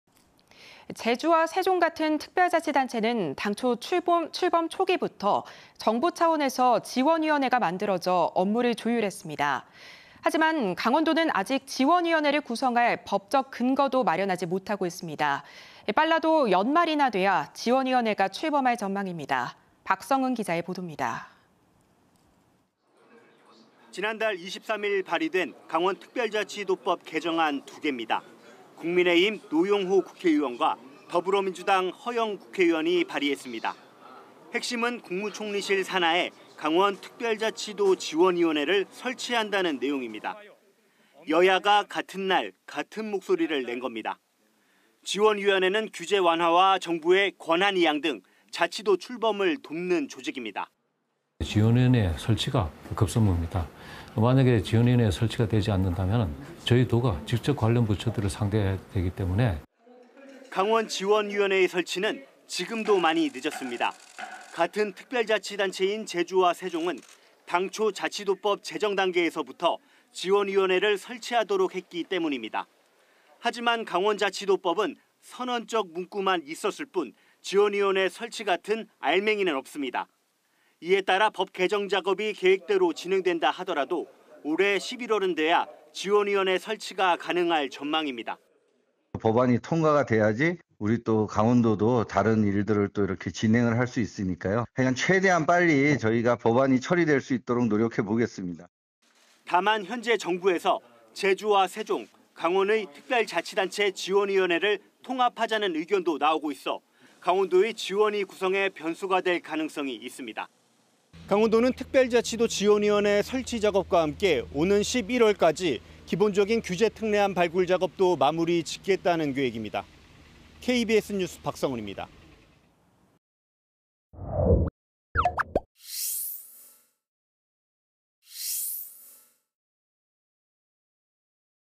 ‘총리실 강원자치도지원위’ 빨라도 11월은 돼야 / KBS 2022.07.25.
[앵커]

 제주와 세종 같은 특별자치단체는 당초 출범 초기부터 정부 차원에서 지원위원회가 만들어져 업무를 조율했습니다.

 하지만 강원도는 아직 지원위원회를 구성할 법적 근거도 마련하지 못하고 있습니다.

 빨라도 연말이나 돼야 지원위원회가 출범할 전망입니다.

 박성은 기자의 보도입니다.

 [리포트]

 지난달(6월) 23일 발의된  강원특별자치도법 개정안 두 개입니다.

 국민의힘 노용호 국회의원과 더불어민주당 허영 국회의원이 발의했습니다.

 핵심은  국무총리실 산하에 강원특별자치도지원위원회를 설치한다는 내용입니다.

 여야가 같은 날, 같은 목소리를 낸 겁니다.

 지원위원회는 규제 완화와 정부의 권한 이양 등 자치도 출범을 돕는 조직입니다.

 [김상영/강원특별자치도추진단 담당관 : "지원위원회 설치가 급선무입니다. 만약에 지원위원회 설치가 되지 않는다면, 저희 도가 직접 관련 부처들을 상대해야 하기 때문에…."]

 강원지원위원회의 설치는 지금도 많이 늦었습니다.

 같은 특별자치단체인 제주와 세종은 당초 자치도법 제정 단계에서부터 지원위원회를 설치하도록 했기 때문입니다.

 하지만 강원자치도법은 '선언적' 문구만 있었을 뿐 지원위원회 설치 같은 알맹이는 없습니다.

 이에 따라  법 개정 작업이 계획대로 진행된다 하더라도, 올해 11월은 돼야  지원위원회 설치가 가능할 전망입니다.

 [노용호/국민의힘 국회의원 : "법안이 통과가 돼야지 우리 강원도도 다른 일들을 진행을 할 수 있으니까요. 하여간 최대한 빨리 저희가 법안이 처리될 수 있도록 노력해 보겠습니다."]

 다만, 현재 정부에서  제주와 세종, 강원의 특별자치단체 지원위원회를 통합하자는 의견도 나오고 있어 강원도의 지원위 구성에 변수가 될 가능성이 있습니다.

 강원도는 특별자치도 지원위원회 설치 작업과 함께 오는 11월까지 기본적인 규제 특례안 발굴 작업도 마무리 짓겠다는 계획입니다.

 KBS 뉴스 박성은입니다.

 촬영기자:임강수